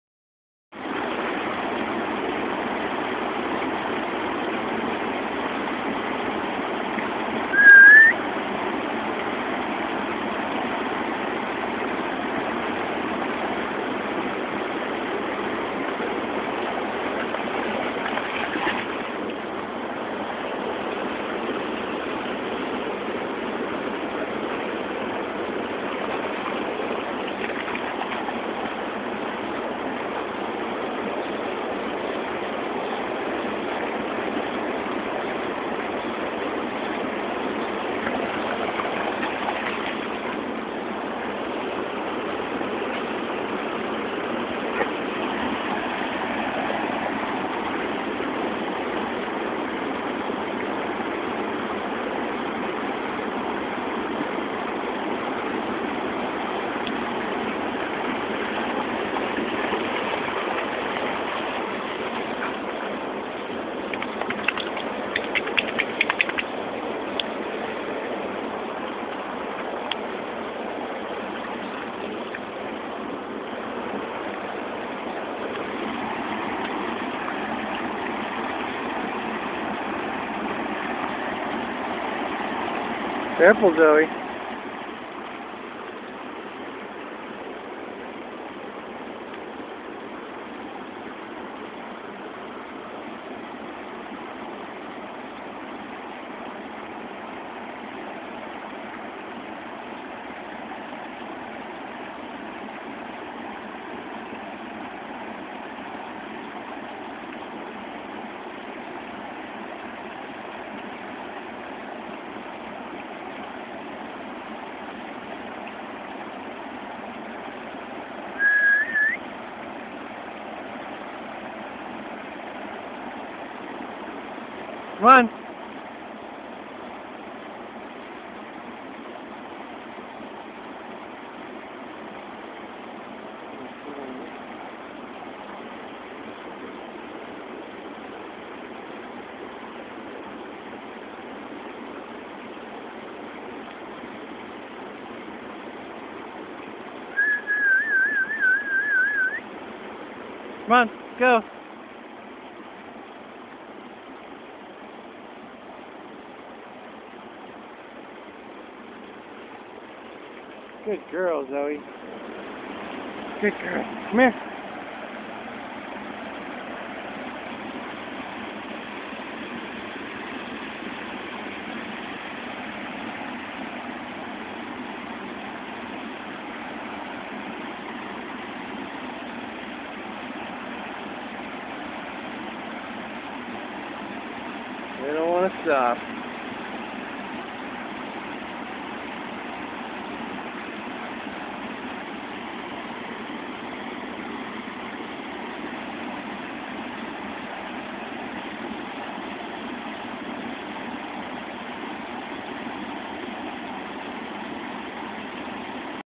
Wakarusa Waterfall
During a visit to the off-leash dog park, Zoe and Bela play in the Wakarusa river near the waterfall.